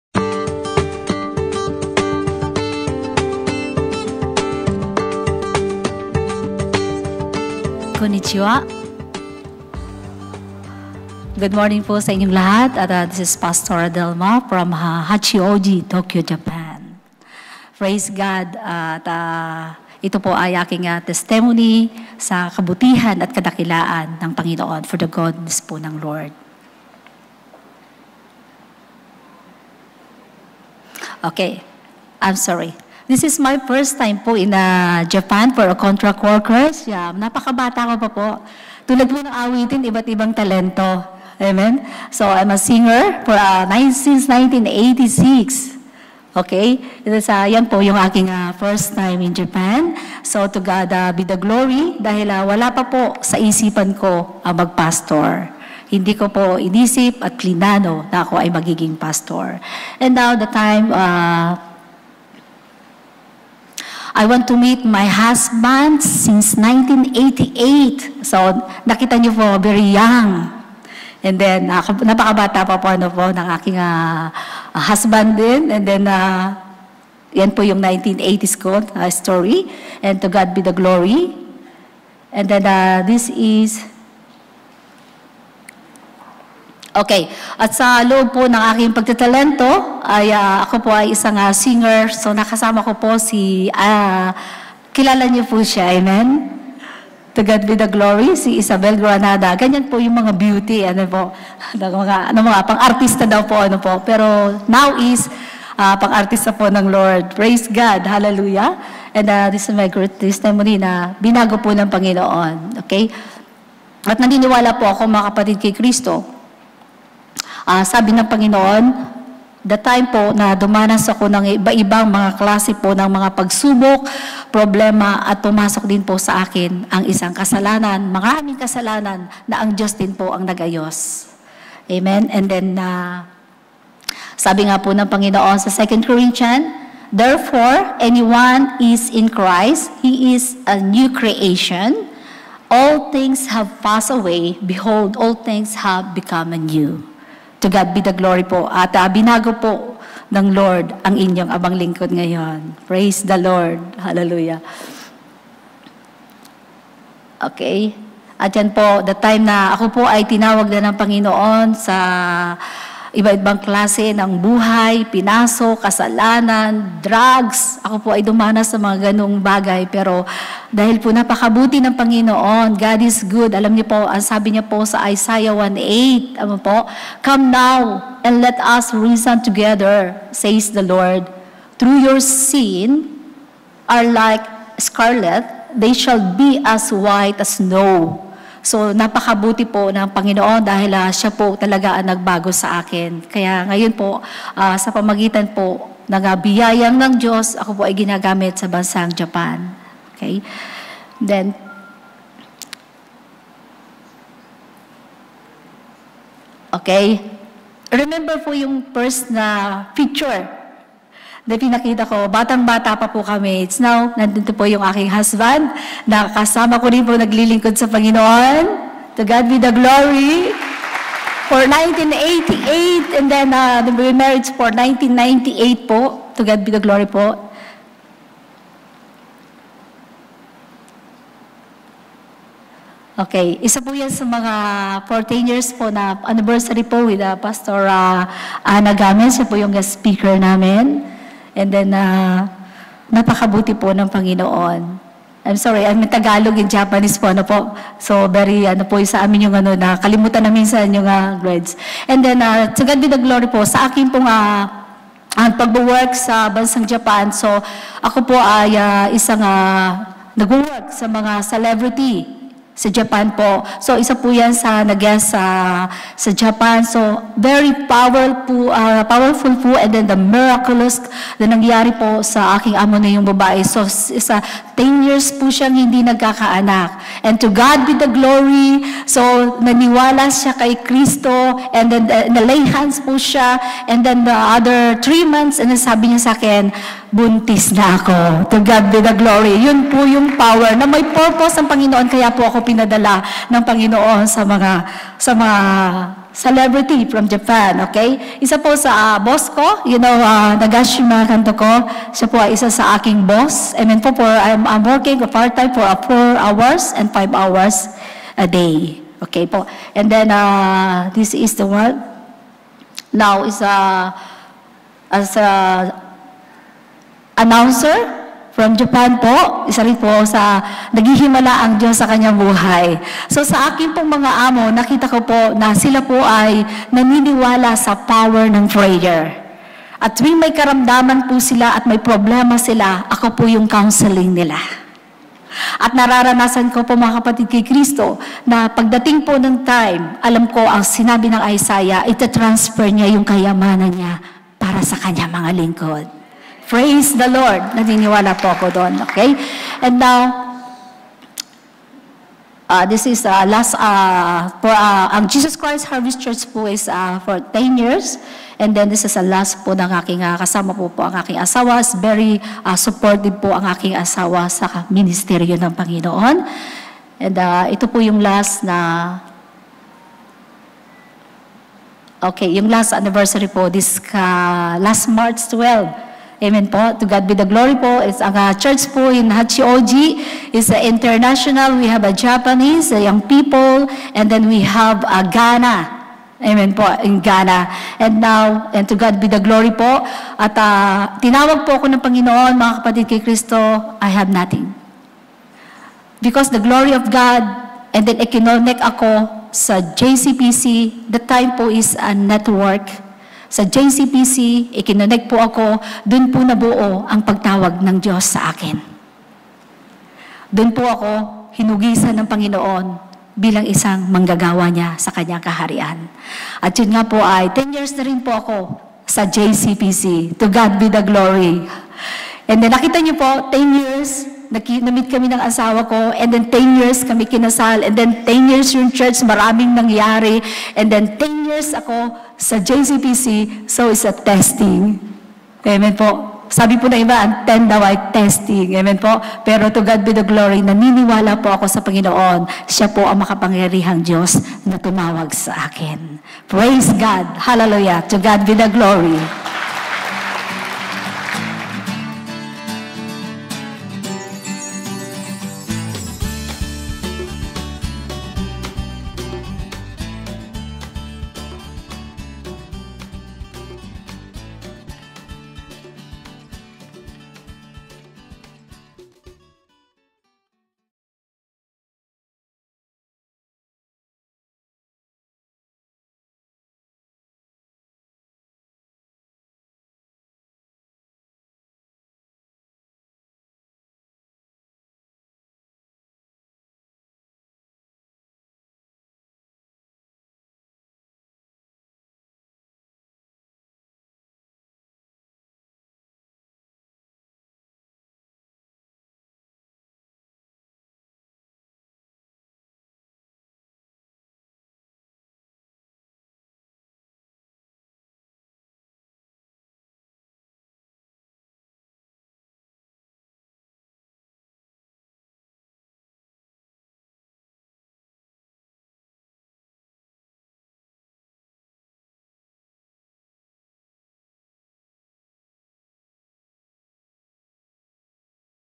BJC Testimony 1 Delma Hashizume
Bless Japan Missions Conference

When : 8 April 2017
Where : Union Church of Manila
Host : Japan Council of Philippine Churches / PMA / PCEC / SEND / OM / WEC / OMF / Megumi

Camera : Claire